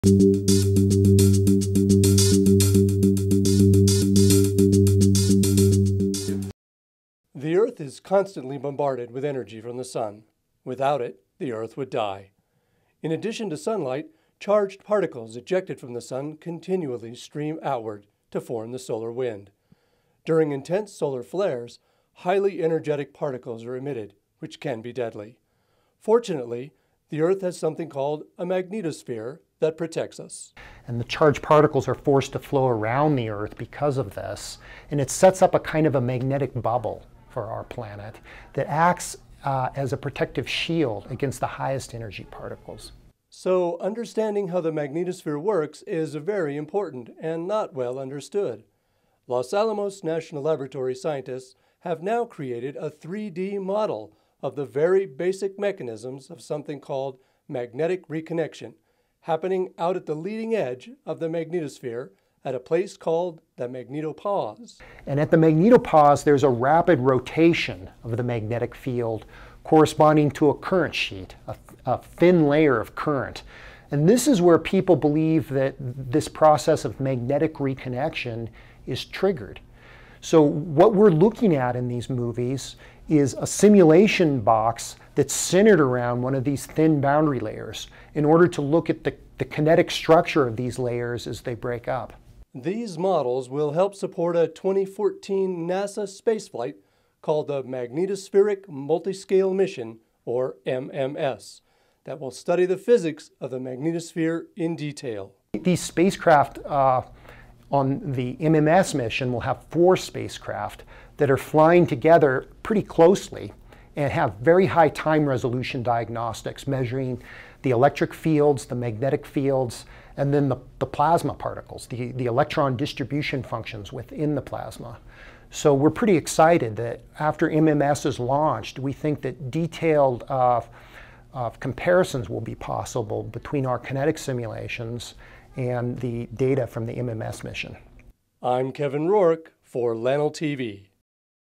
LANL Studies Earth's Magnetosphere
A new 3-D supercomputer model presents a new theory of how magnetic
reconnection works in high-temperature plasmas.  This Los Alamos National Laboratory research supports an upcoming NASA mission to study Earth's magnetosphere in greater detail than ever.